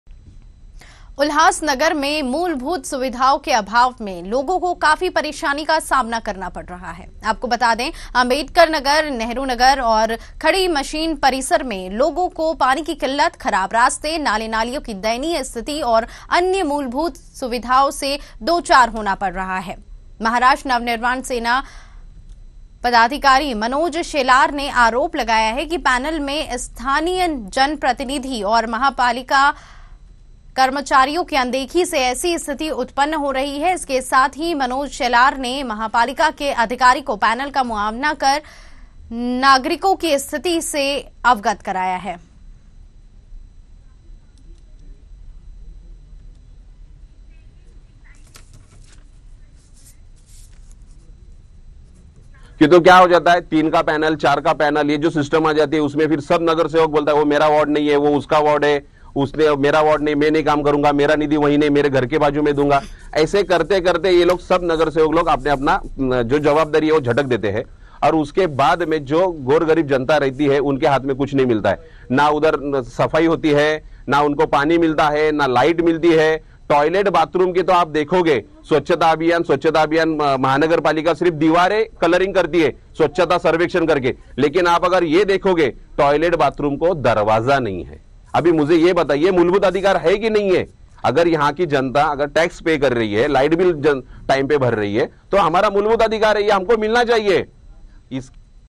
उल्हासनगर में पैनल 20 के नागरिक मूलभूत सुविधाओं से वंचित || MKN NEWS CHANNEL
उल्हासनगर में पैनल नंबर  20 के आंबेडकर नगर , नेहरू नगर खडी मशीन परिसर में कई दिनों से मूलभूत सुविधाओं का अभाव है. पीने का पानी की किल्लत, खराब रास्ते, नाले-नालियों की दयनीय स्थिति और अन्य मूलभूत सुविधाओं की कमी से स्थानिक निवासी परेशान हैं। पैनल में स्थानिक जनप्रतिनिधी और महापालिका कर्मचारियों की अनदेखी से ऐसी स्थिती उत्पन्न होने का आरोप महाराष्ट्र नवनिर्माण सेना पदाधिकारी मनोज शेलार ने किया. मनोज शेलार ने महापालिका के अधिकारी को पैनल का मुआयना करवाकर नागरिकों की स्थिति से अवगत कराया। वहीं समस्याओं का जल्द निदान नहीं निकलने पर आंदोलन का इशारा दिया।